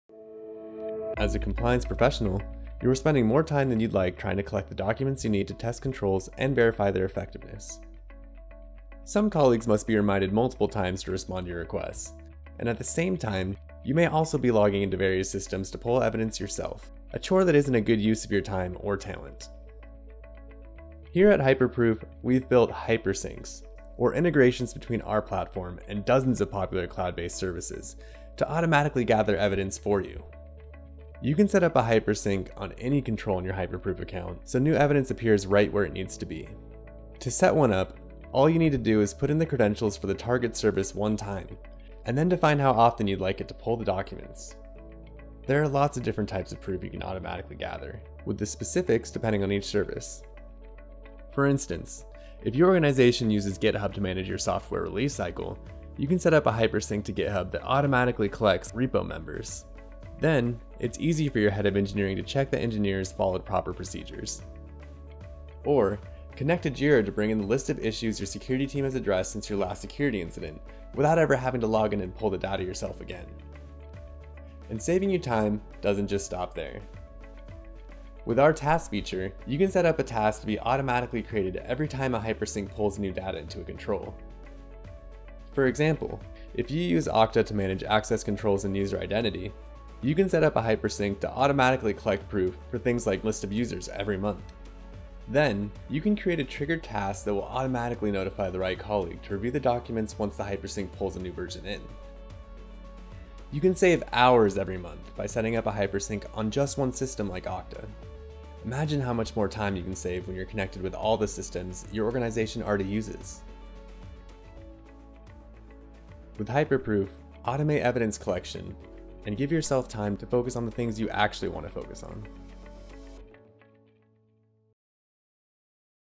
Automate Evidence Collection with Hypersync in Hyperproof
Hypersync allows you to automate the collection of proof from AWS, Azure, Github, and other applications and store proof files directly on a Hyperproof control, in your proof repository, or on a specific label. You can define the cadence for automated collection based on your organization’s needs.

By setting up automated proof collection from AWS, Azure, and GitHub, you can automatically collect the following types of evidence from these systems: Backup settings, encryption settings, access groups and lists of users, code change management evidence, and more. Getting these types of proof automatically can save a lot of time as you prepare for assessments or information security framework audits.